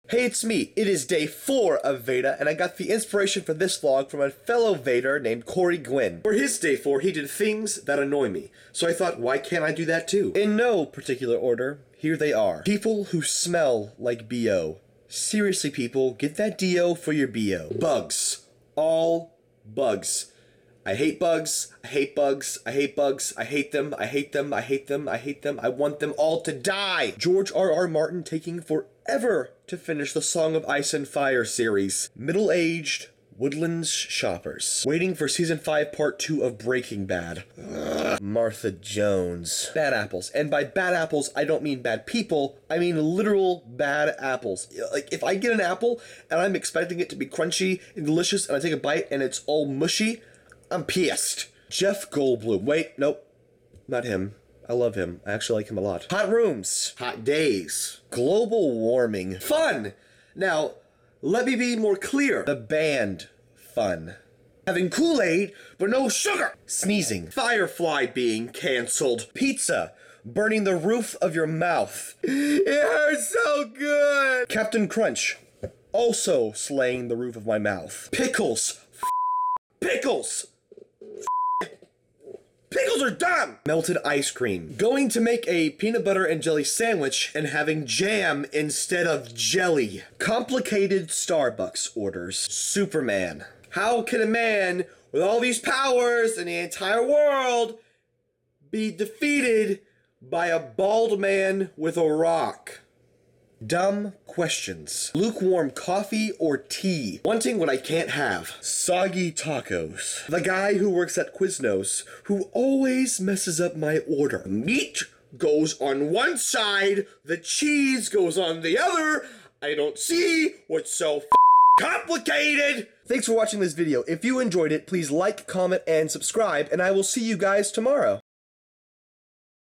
VEDA Day Four: Annoying Pickles
These are the things that annoy me. Maybe some of them annoy you. Maybe I annoy you.